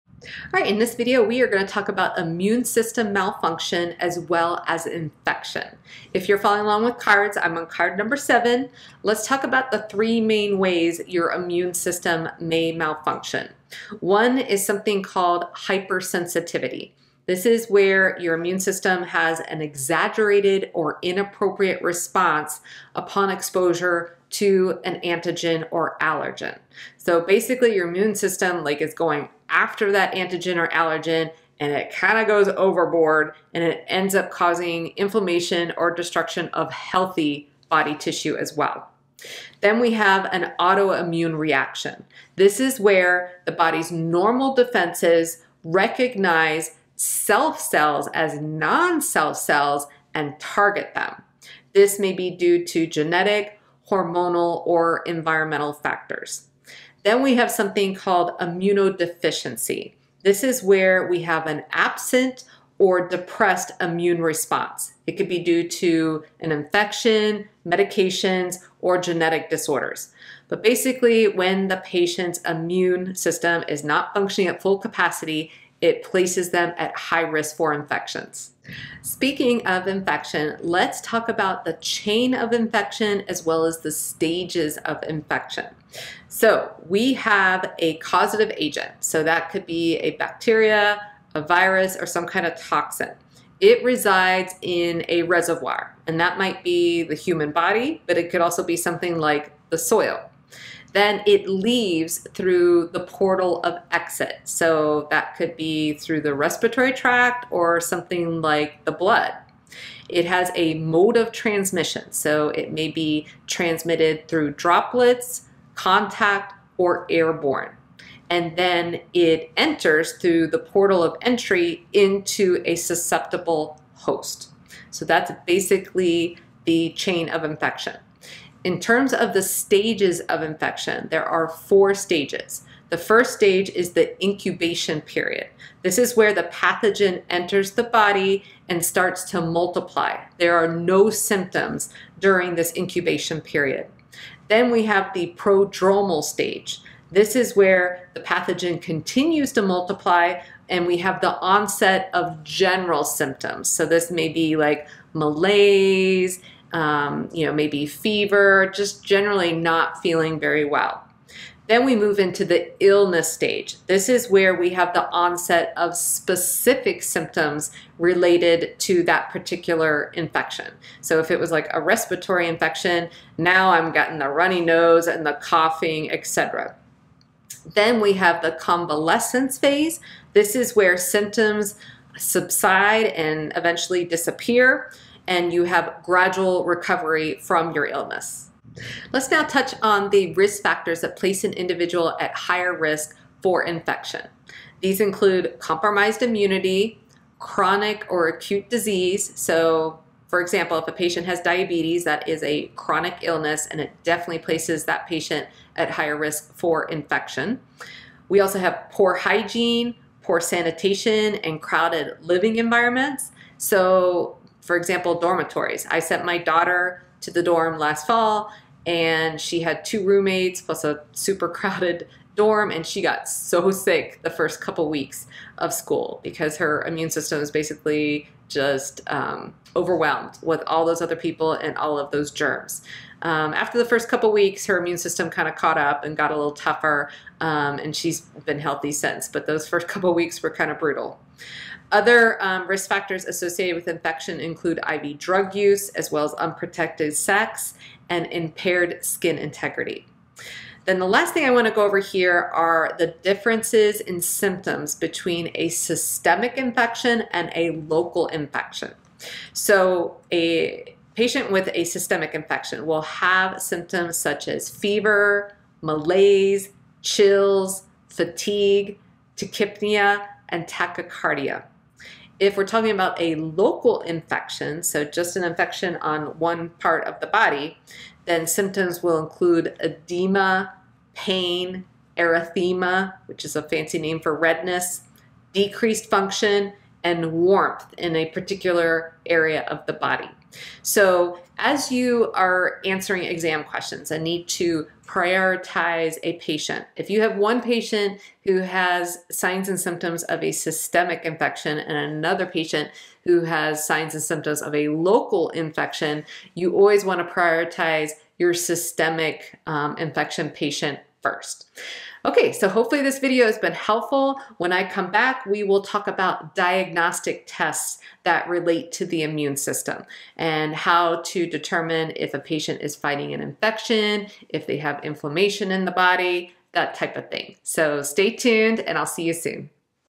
Immune System Malfunction & Infection: Medical-Surgical - Immune System
Cathy covers key types of Immune System malfunction, including: hypersensitivity, autoimmune reactions, and immunodeficiency. She also discusses infection, including the chain of infection, stages of infection, risk factors associated with infection, and symptoms of systemic vs. local infections.

0:00 Immune system malfunction
0:13 Hypersensitivity
0:39 Autoimmune Reaction
0:57 Immunodeficiency
1:18 Infection
1:50 Chain of infection
2:08 Stages of infection
3:09 Risk factors
3:20 Example
4:01 Other Risk Factors
4:21 Systemic Infections
4:32 Local Infections
4:50 Priority for Treatment
5:12 What’s Next?